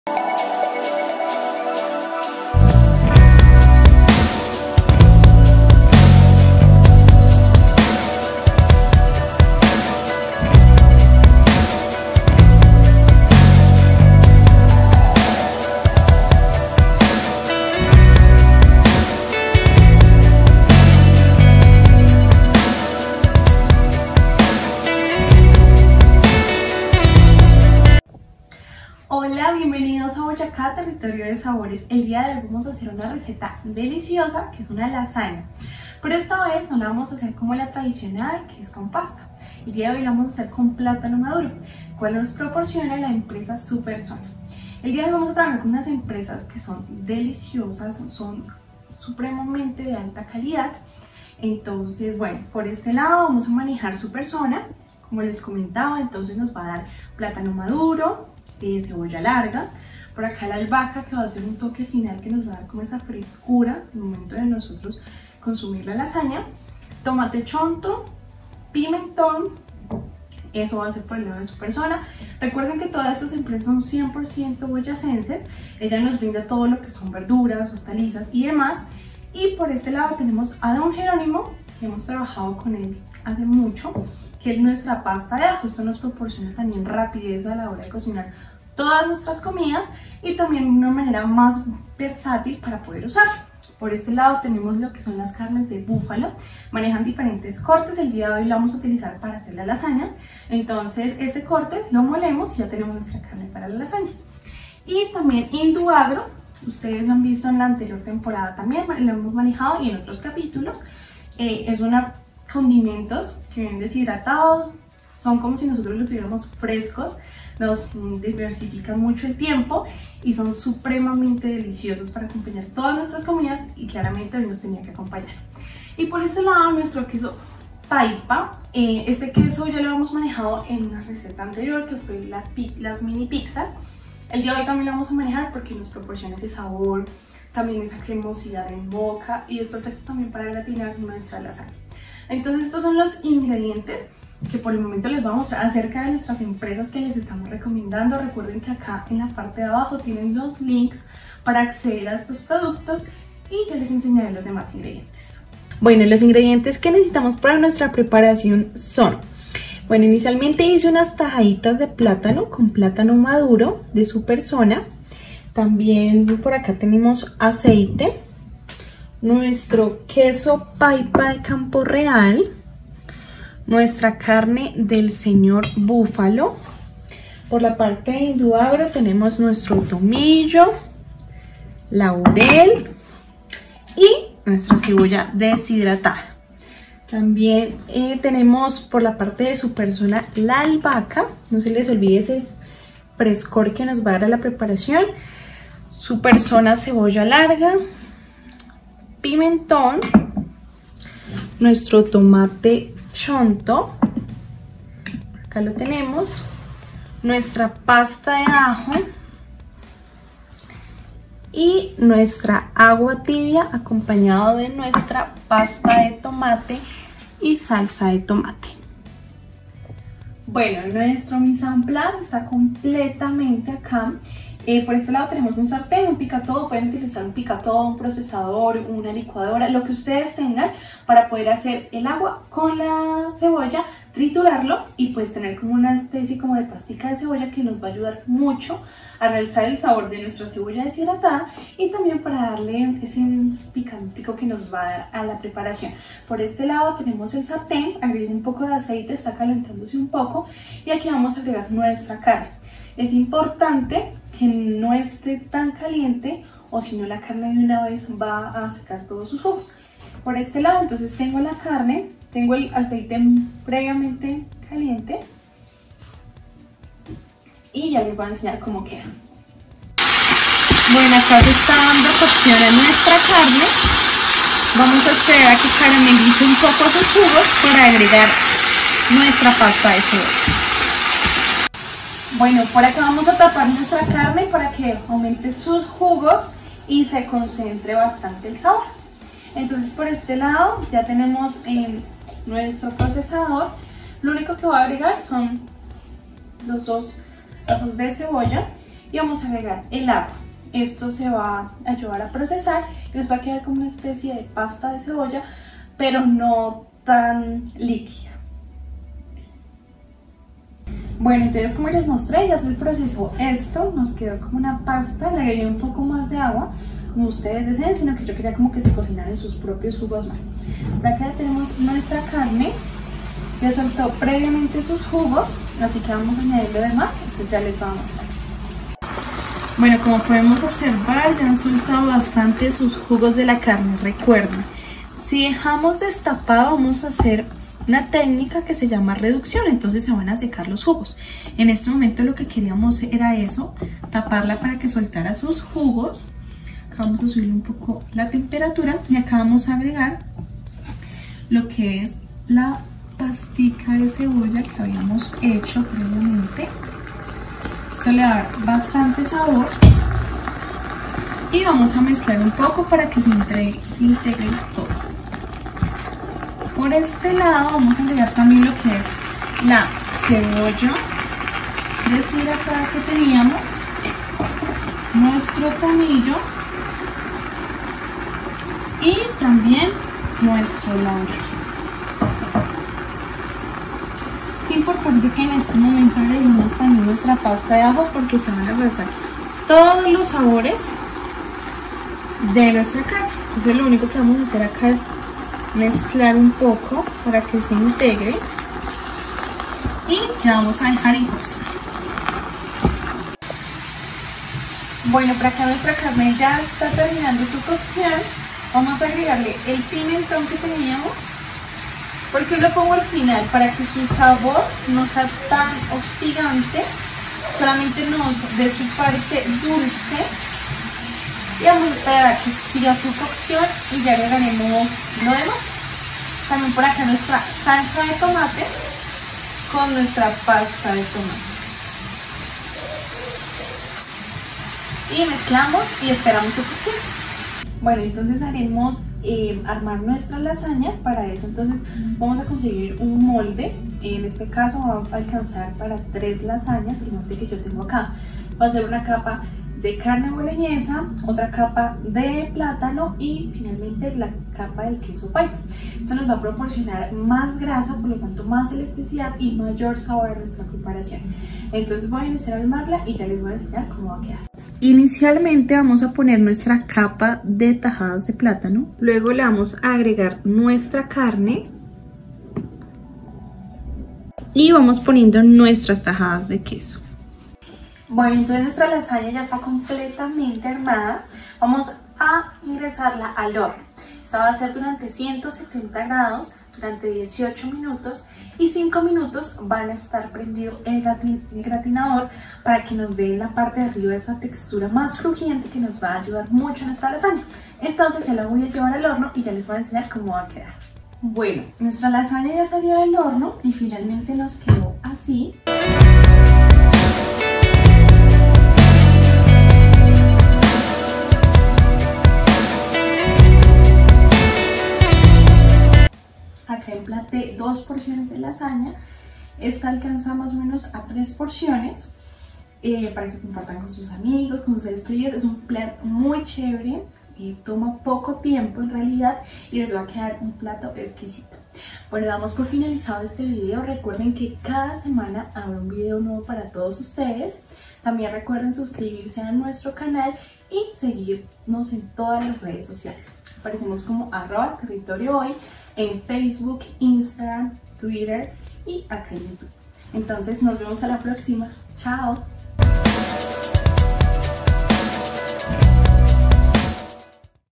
Lasagna de Platano - Cocinando con Boyacá Territorio de Sabores
El día de hoy queremos ayudarte a preparar una deliciosa "Lasagna de Platano" ideal para realizar fácil, rápido y sobre todo disfrutar haciéndolo.
Ideado por nuestra chef  invitada Valentina Castillo.
Receta:
Albahaca Superzona 2 gr
Plátano maduro 2 unidades Superzona (tajadas previamente freídas)
Pimentón Superzona 45 gr
Tomate Chonto Superzona 30 gr
Cebolla larga Superzona 14 gr
Cebolla deshidratada Induagro 9 gr
Tomillo Induagro 8 gr
Laurel Induagro 5 gr
Salsa de tomate 110 gr
Agua tibia 100 ml
 Pasta de tomate 40 gr
Aceite 30 ml
Pasta de ajo 6 gr

Muéstranos que tal quedo tu receta.